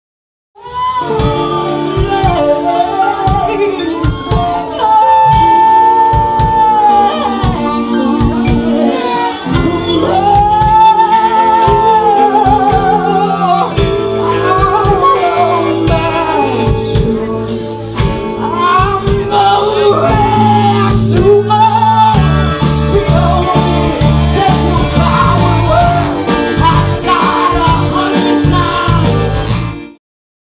ALEOproduction: QOS Promo
- Queens of Soul - with:
Christina Alessi
Casey Solomon
Jaime DellaFave
Carolyn Monroe
Sylvana Joyce

On February 24, 2016, join these magnificent women, backed by a killer band, performing different genres of "soul" music, from Motown - Cosmic Gospel.
Maxwell's - Hoboken, NJ

Filmed and edited by Chris Capaci of Capacity Images